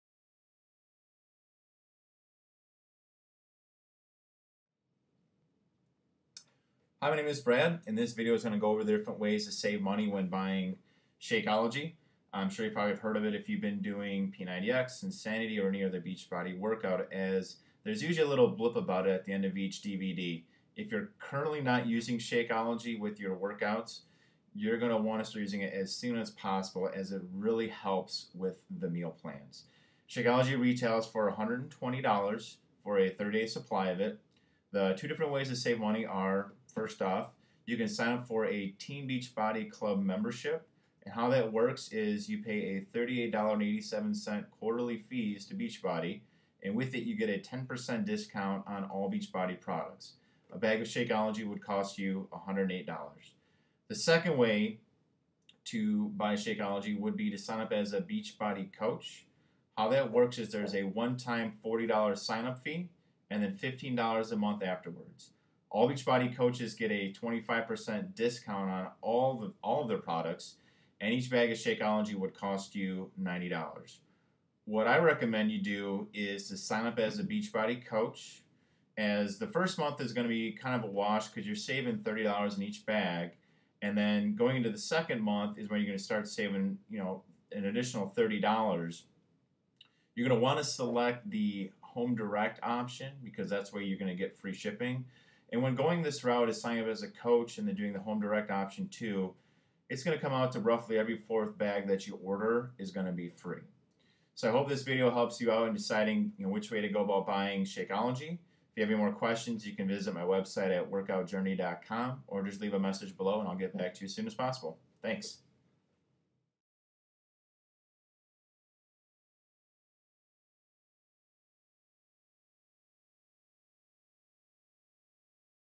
Buy Shakeology Cheap
Learn the different ways to buy Shakeology cheap from a Beachbody Coach.